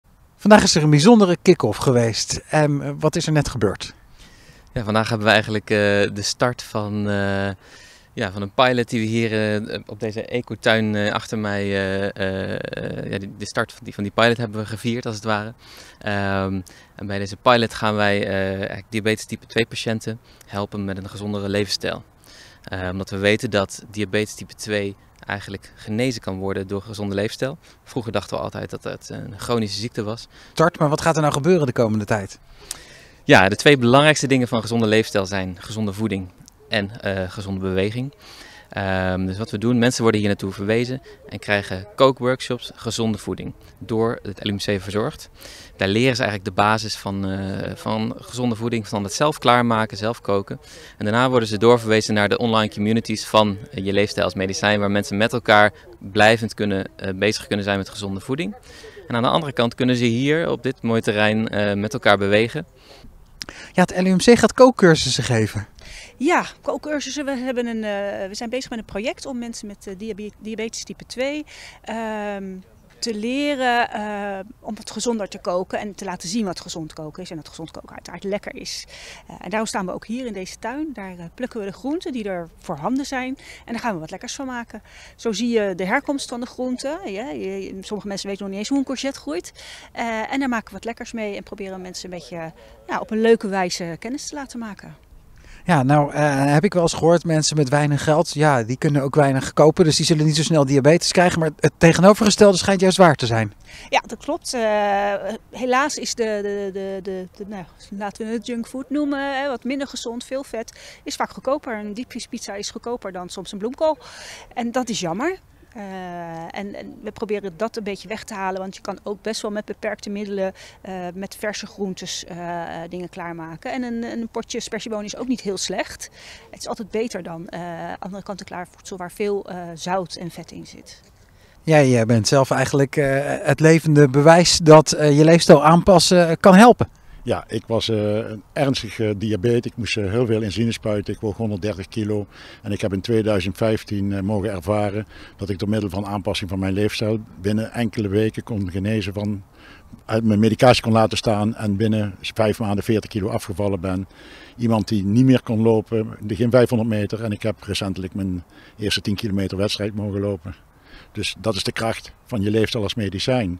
Leefstijlprogramma tegen diabetes type 2
Bij kwekerij de Ecotuin start vandaag een pilot met een leefstijlprogramma dat mensen met diabetes type 2 helpt om duurzaam gezonder te leven. Uit onderzoek blijkt dat mensen hun diabetes kunnen ‘omkeren’ door meer te bewegen en gezonder te eten, maar dat het ook moeilijk is om gedrag te veranderen en dit vol te houden.